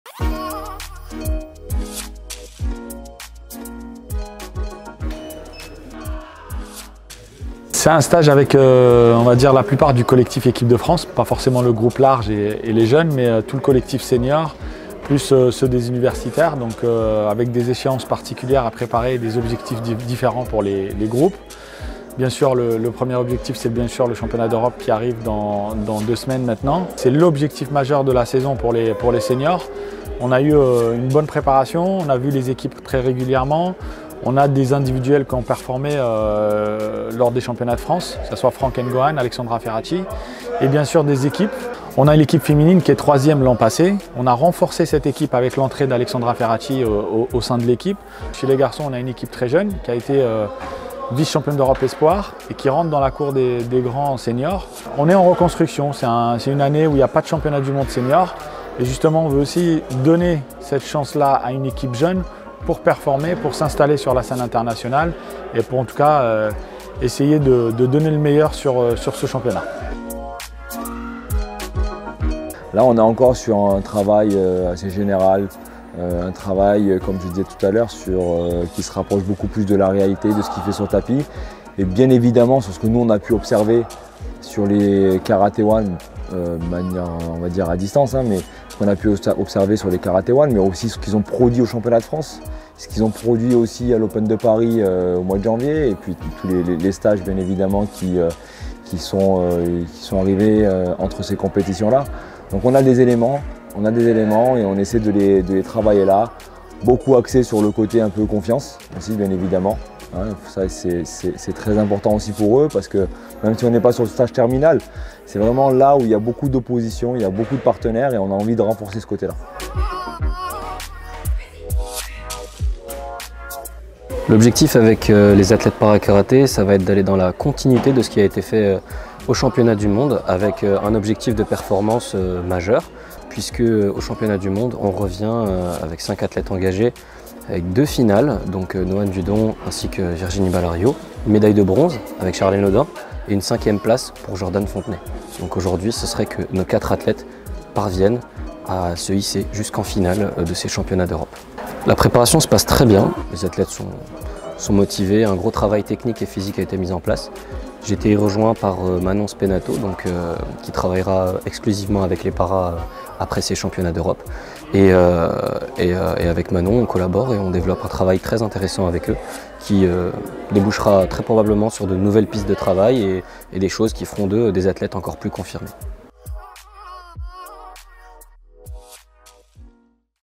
Championnats d'Europe : la préparation continue !
À deux semaines du début des Championnats d'Europe, le collectif France Karaté se réunissait au Centre National d'Entraînement pour parfaire sa préparation.